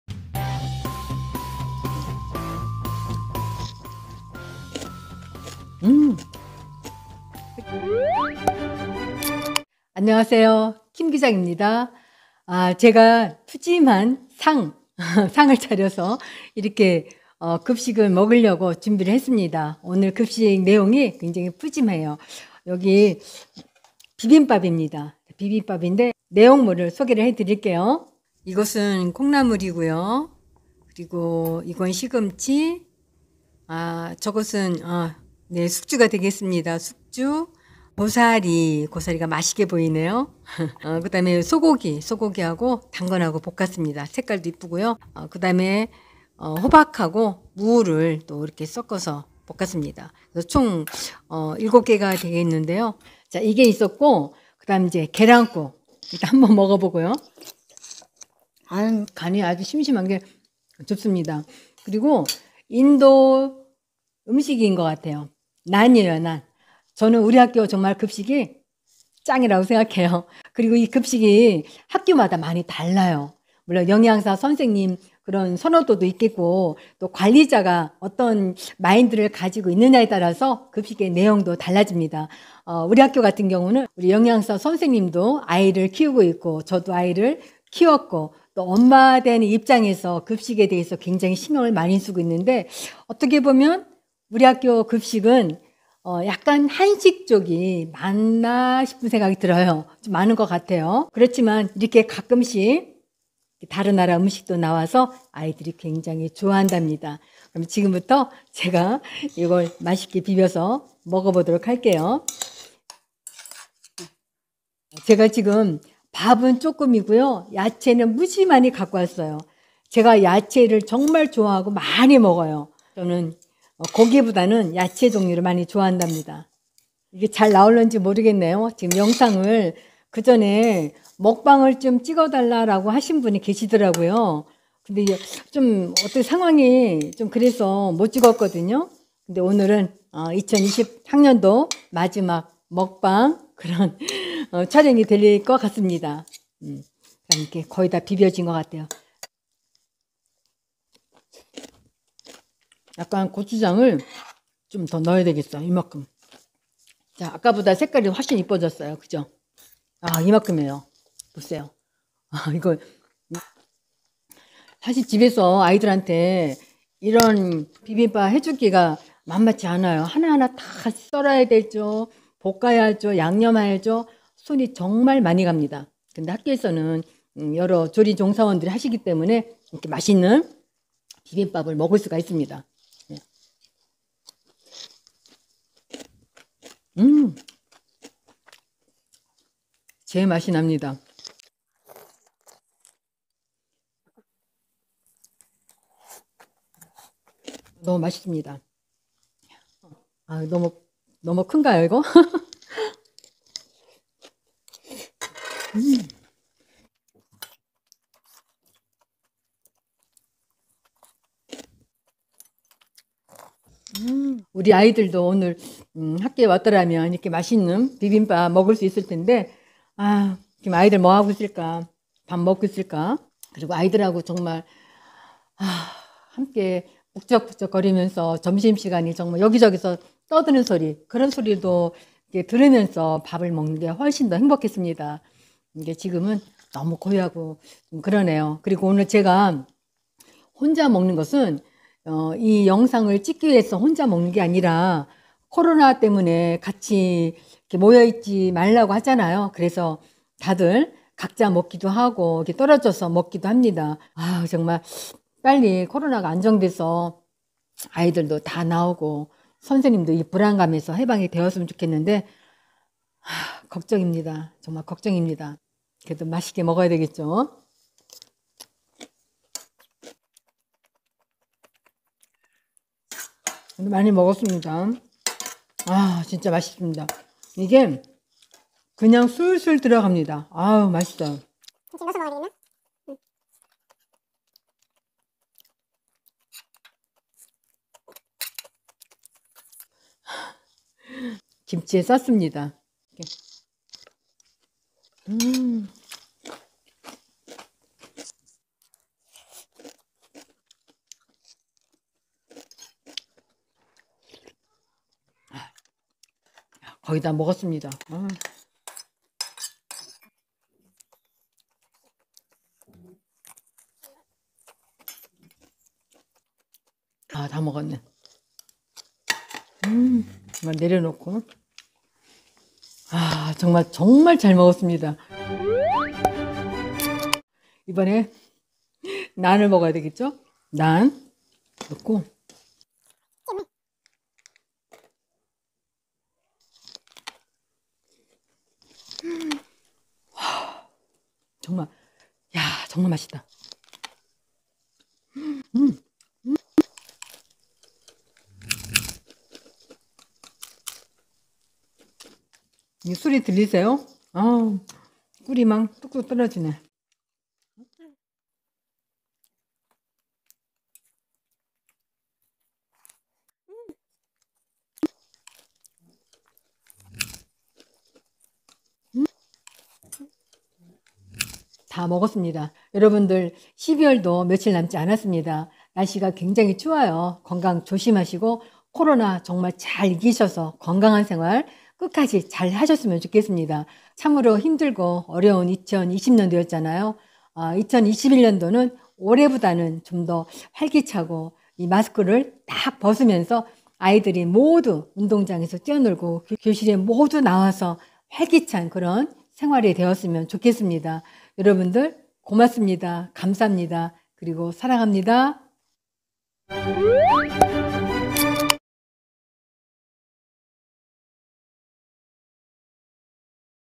초딩들의 엄지척👍끝내주는 학교급식의 결정판, 우리 학교 최고 인기 비빔밥, "더 주세요" 킴교장의 학교급식 먹방
안녕하세요?
김교장 입니다.

갑자스럽게 닥쳤던 코로나로 학교 급식에 대한 관심과 소중함을
더욱 알게 되었습니다.
이번 급식 먹방은 먹방이라기 보다 나빠진 코로나로 갑자기 중단하게 된
급식에 대한 이야기 입니다. 들어 보시고 급식 메뉴가 어떻게 짜여지는지 조금은 아실 수 있을 것 같습니다.
그리고 우리 학교 최고 인기 메뉴 중 하나인 비빔밥은 "원더플 원더플" 외치게 하는 아주 끝내주는 비빔밥이랍니다.

여러분,
이제 12월도 얼마 남지 않았네요.
일 년동안 아이들과 우리 학부모님들 고생 많으셨습니다.
좀 더 나은 2021년도를 기대하면서 사랑가득한 남은 시간 보내시길 바랍니다.
감사합니다.